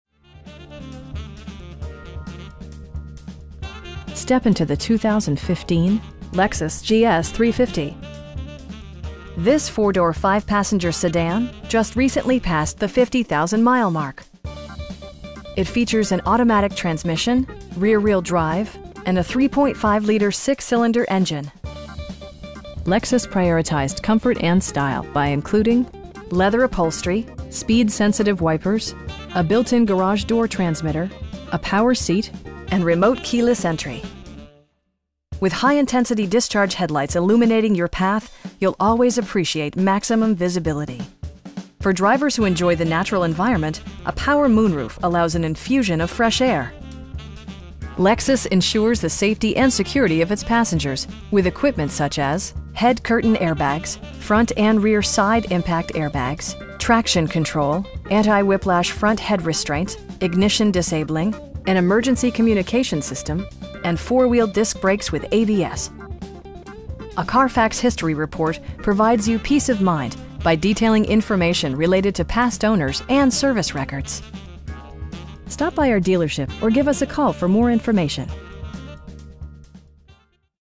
2015 Lexus GS 350 4dr Sdn RWD in Atlanta, GA 30341
World Toyota
5800 Peachtree Industrial Blvd.
in Atlanta, GA 30341

Step into the 2015 Lexus GS 350. This 4 door, 5 passenger sedan just recently passed the 50,000 mile mark! It features an automatic transmission, rear-wheel drive, and a 3.5 liter 6 cylinder engine. Lexus prioritized comfort and style by including: leather upholstery, speed sensitive wipers, a built-in garage door transmitter, a power seat, power windows, an overhead console, and remote keyless entry. With high intensity discharge headlights illuminating your path, you'll always appreciate maximum visibility. For drivers who enjoy the natural environment, a power moon roof allows an infusion of fresh air. Lexus ensures the safety and security of its passengers with equipment such as: head curtain airbags, front and rear side impact airbags, traction control, brake assist, anti-whiplash front head restraints, ignition disabling, an emergency communication system, and 4 wheel disc brakes with ABS. Various mechanical systems are monitored by electronic stability control, keeping you on your intended path. A Carfax history report provides you peace of mind by detailing information related to past owners and service records. We have the vehicle you've been searching for at a price you can afford. Stop by our dealership or give us a call for more information.